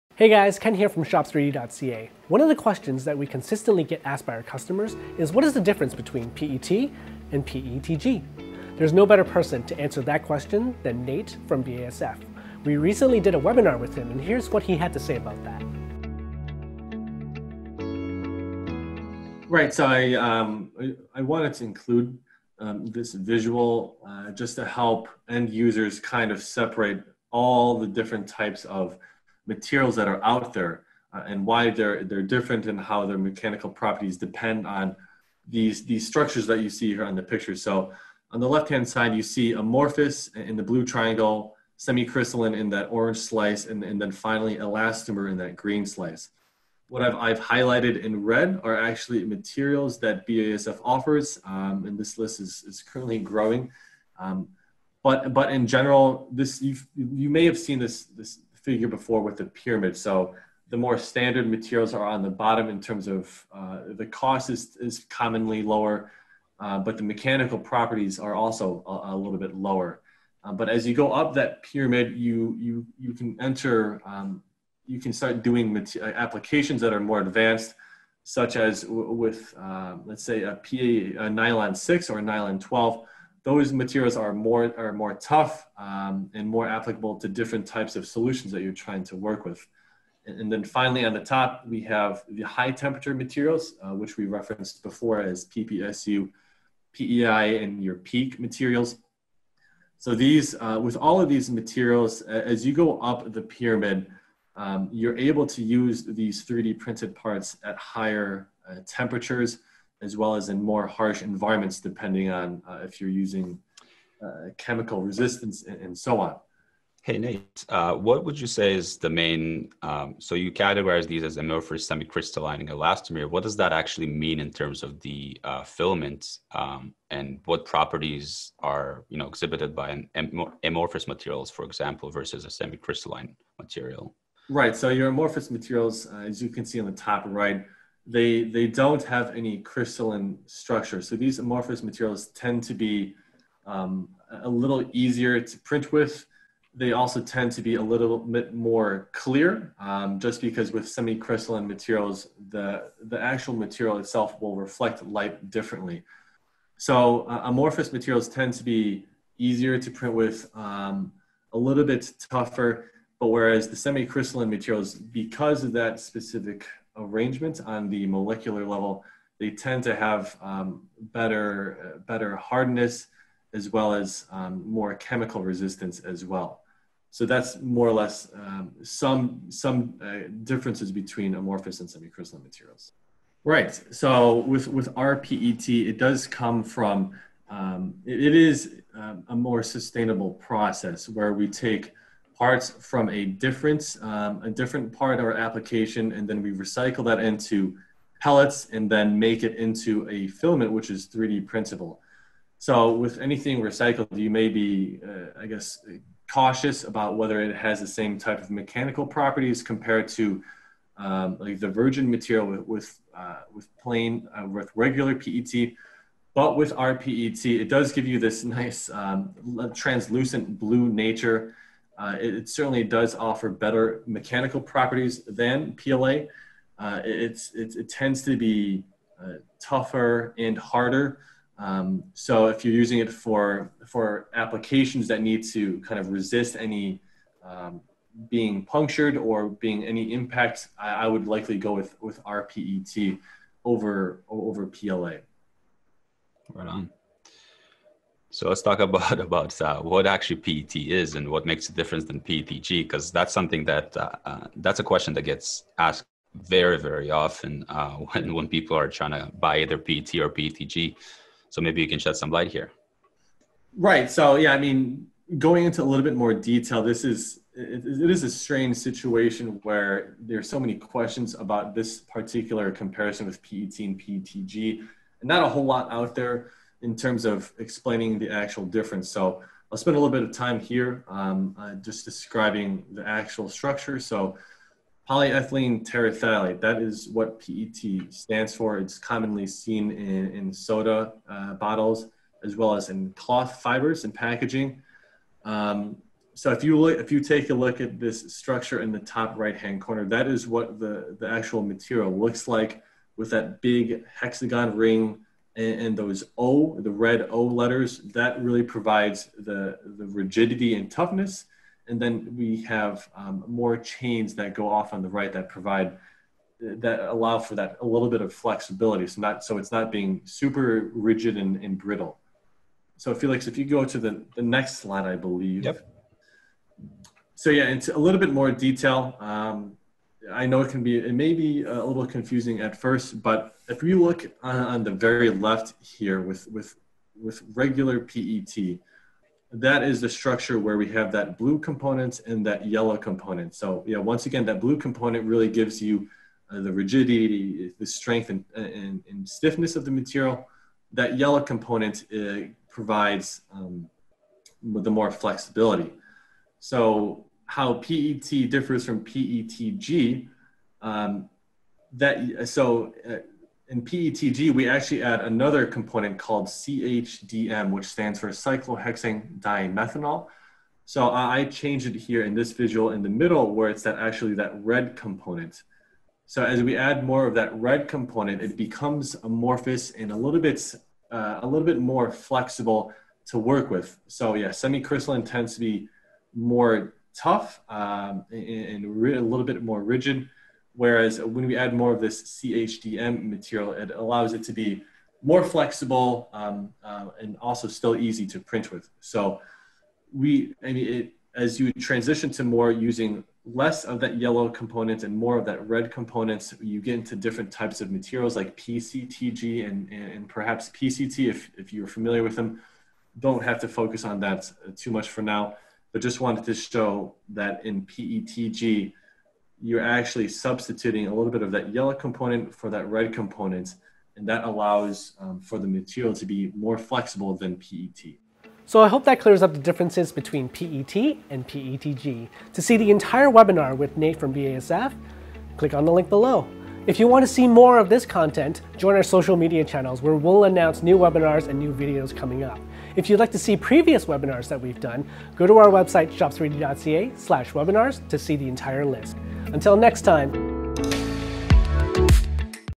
Summary and PET PETG Comparison
Did you ever wonder what the exact difference between PET and PETG is? Stiff or flexible? Easy to print or hard?

Hear from Nate Wood, a chemical expert from BASF’s Forward AM group about the key differences between PET and PETG. Nate discusses the molecular differences and their effect on the 3D printed end product.